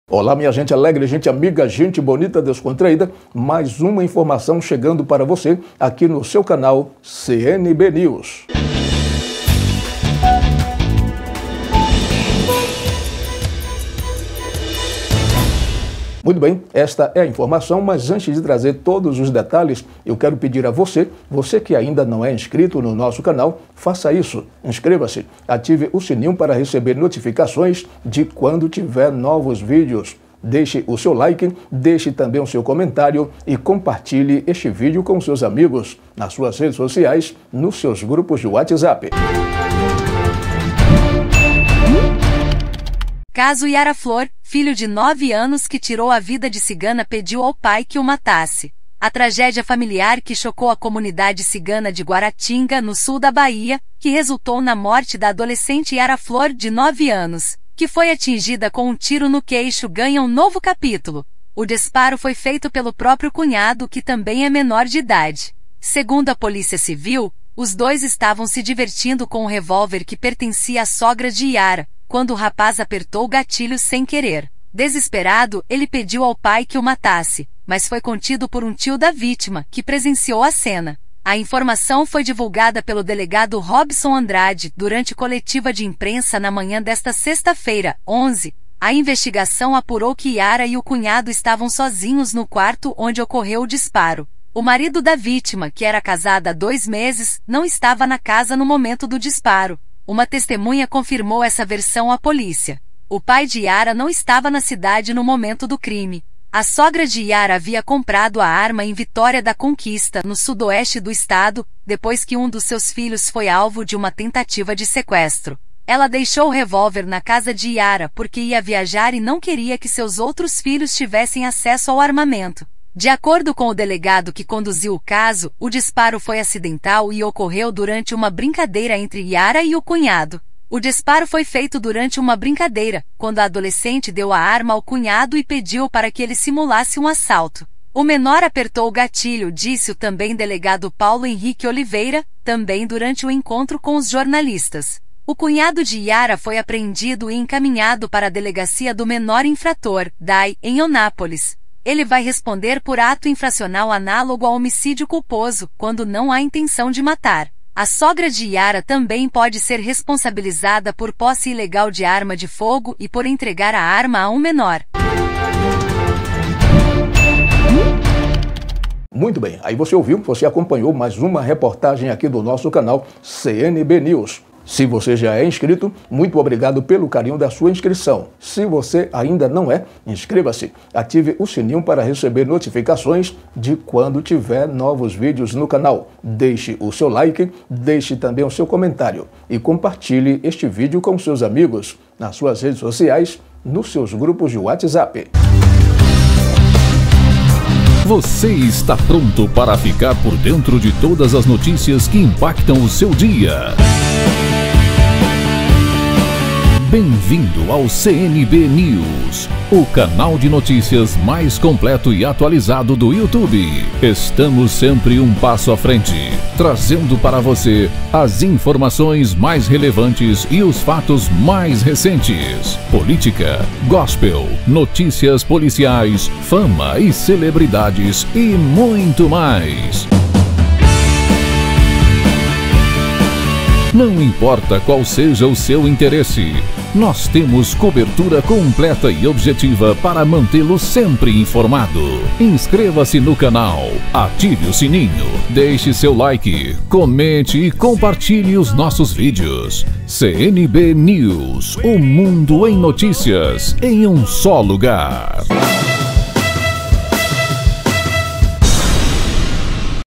CASO HYARA: CUNHADO DE 9 ANOS QUE TIROU A VIDA DE CIGANA, PEDIU AO PAI QUE O MATASSE
A tragédia familiar que chocou a comunidade cigana de Guaratinga, no sul da Bahia, que resultou na morte da adolescente Hyara Flor, de 9 anos, que foi atingida com um tiro no queixo, ganha um novo capitulo. O disparado foi feito pelo próprio cunhado, que também é menor de idade. Segundo a Polícia Civil, os dois estavam se divertindo com um revólver que pertencia à sogra de Hyara, quando o rapaz apertou o gatilho sem querer. Desesperado, ele pediu ao pai que o matasse, mas foi contido por um tio da vítima, que presenciou a cena. A informação foi divulgada pelo delegado Robson Andrade, durante coletiva de imprensa na manhã desta sexta-feira (11).